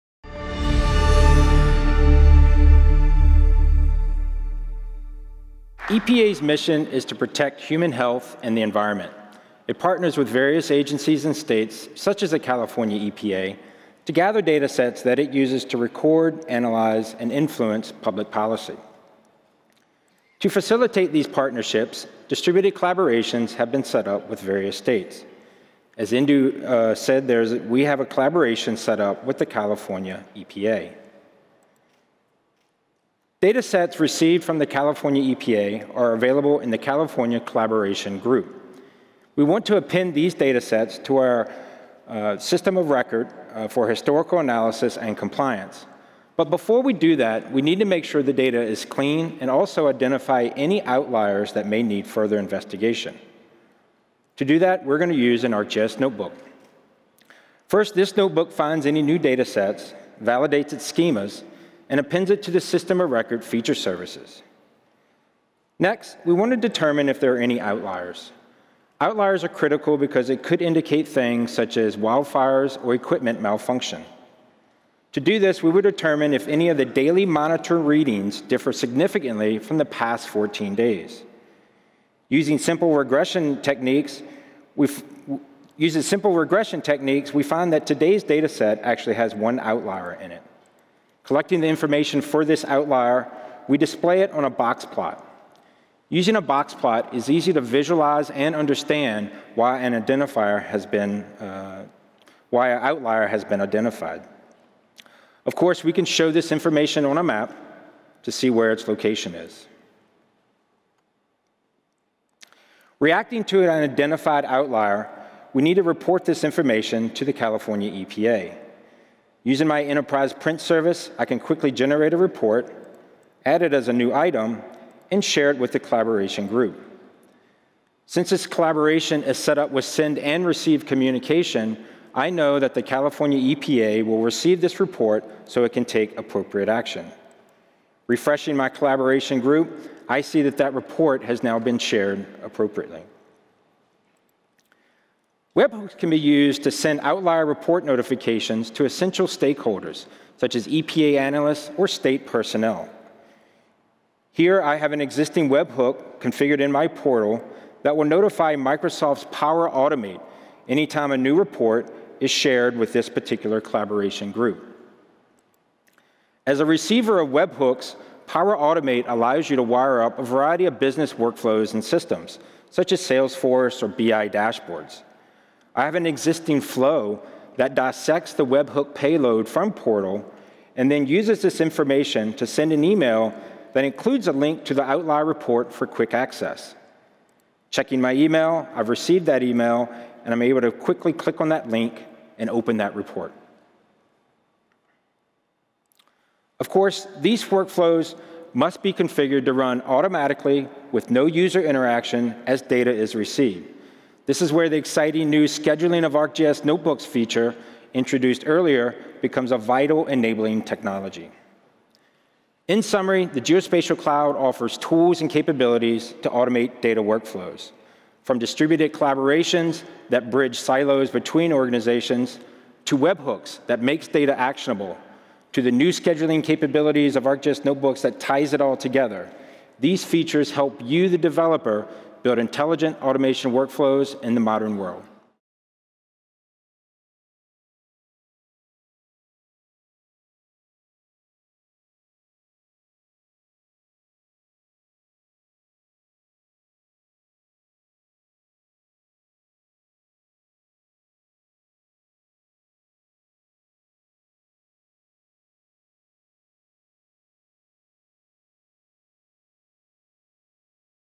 Scheduling Notebooks with ArcGIS Enterprise
Using Python with Notebooks is an effective way to build your analytical projects. Not only can your processes be documented, communicated, and edited easier and well with those you're collaborating with, but are a great way to schedule and automate tasks done often.